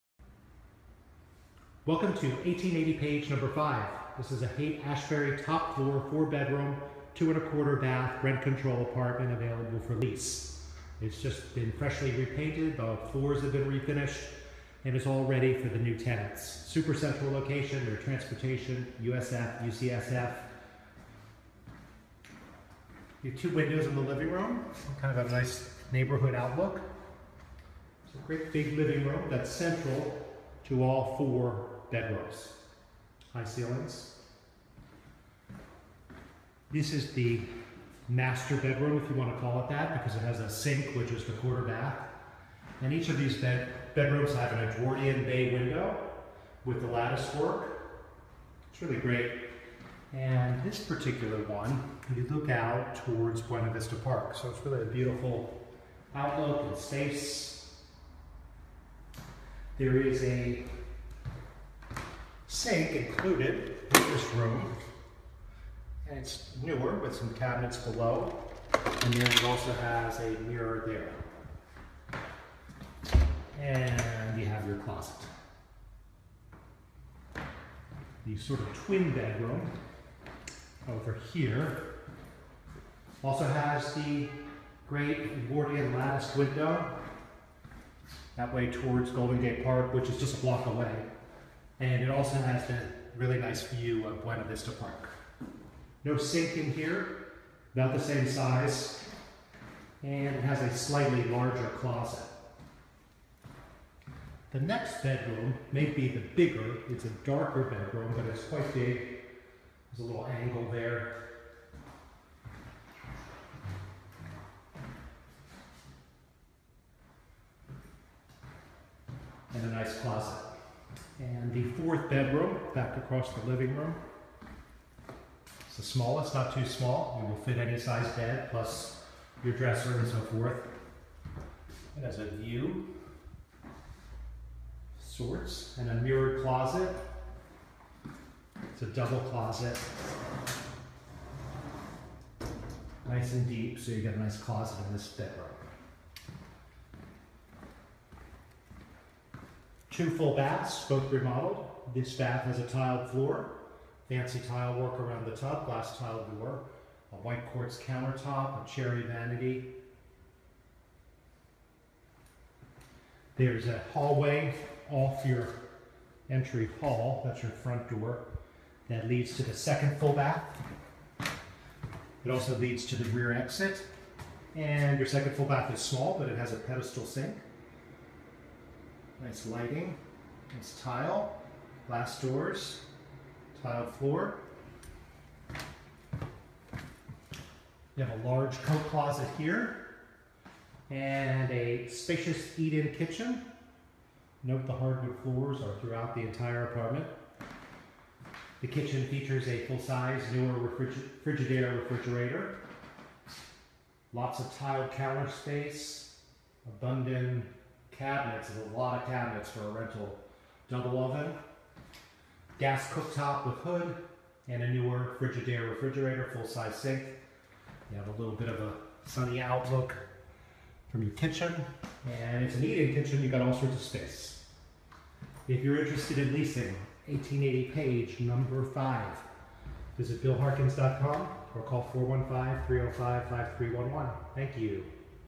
1880 Page Street #5 $5900
1880 Page Street is a stately Haight Ashbury 1908 Edwardian with formal lobby & on-site laundry.

Top Floor apartment #5 is a spacious 4 bedroom 2.25 bath apartment with newly refinished hardwood floors and Edwardian box bay windows in both front bedrooms. All 4 bedrooms are "real bedrooms" with closets & windows. Sunny center living room, large separate kitchen with eating area. The kitchen has been remodeled and features newer Frigidaire refrigerator, gas range with hood, dishwasher and double oven. Two full hall baths, one is all new. One of the bedrooms has a sink inside (1/4 bath).

Unbeatable location 1 block from Golden Gate Park, Whole Foods, USF, UCSF, Panhandle, Haight Street, transportation and tech shuttles.